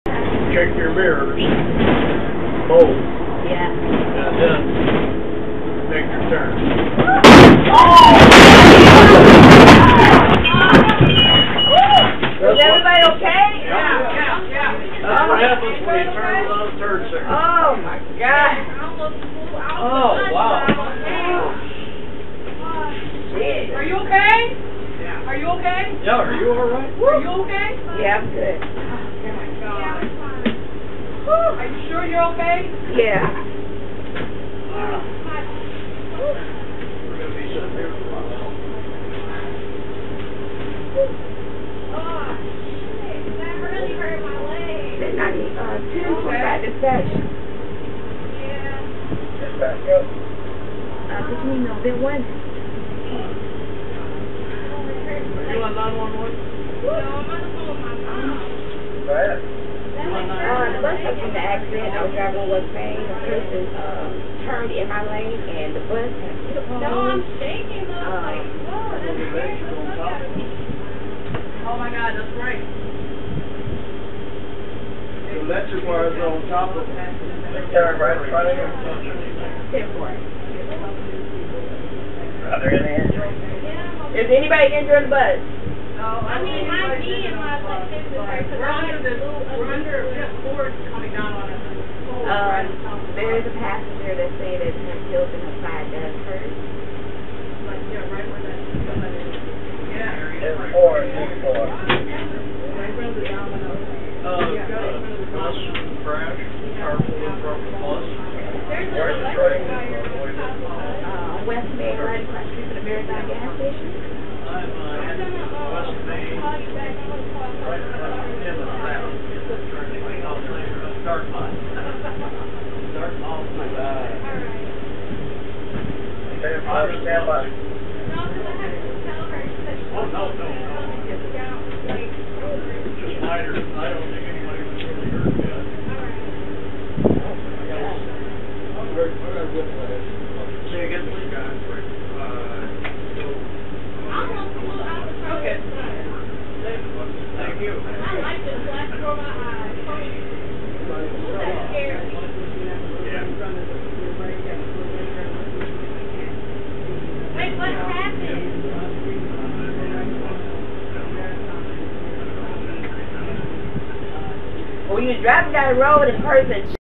Video shows Kalamazoo Metro Transit bus hit utility pole
Video, obtained by 24 Hour News 8 on Monday through the Freedom of Information Act, shows seven working cameras on the bus. (May 11, 2017)